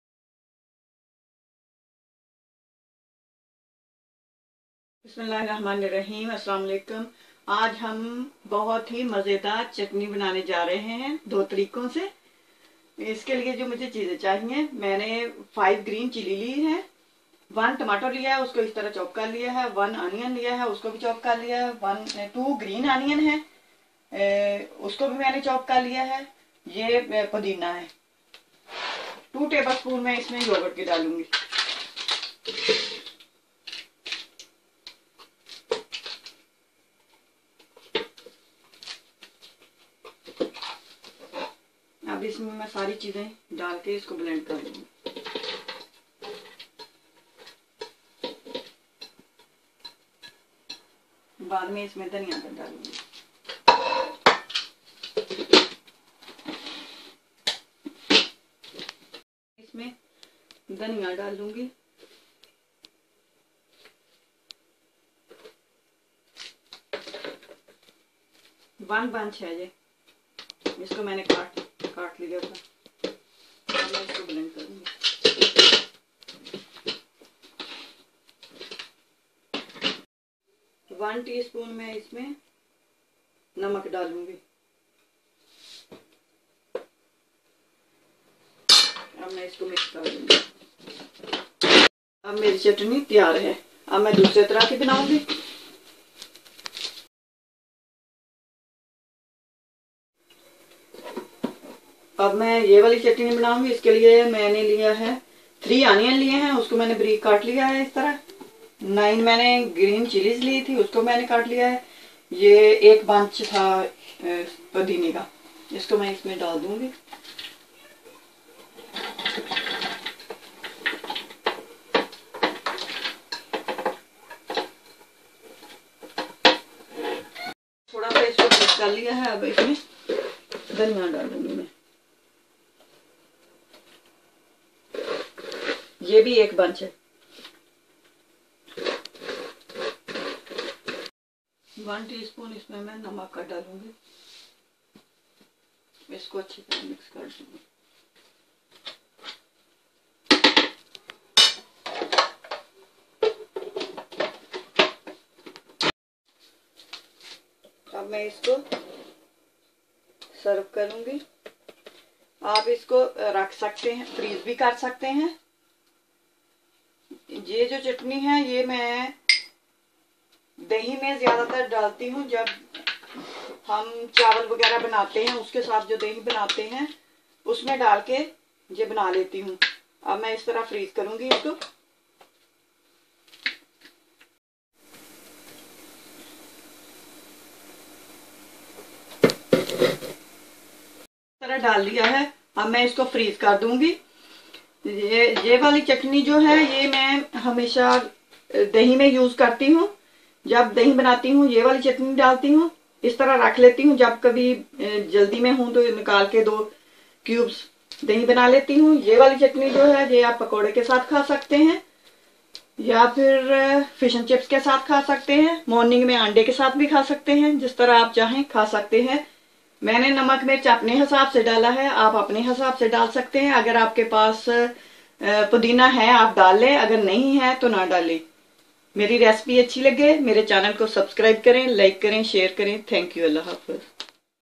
Green Chutney Recipe
Welcome to cooking with Abida, today I will be showing you how to make 2 different types of green chutney.
Ingredients:

chutney 1
- 1 onion
- 1 tomato
- 5 green chillies
- 2 spring onions
- 2 tbsp yogurt
- 1 tsp salt
- 4 tbsp mint
- 1 bunch of coriander

chutney 2
- 3 onions
- 9 green chillies
- 1 tsp salt
- 1 bunch of mint
- 1 bunch of coriander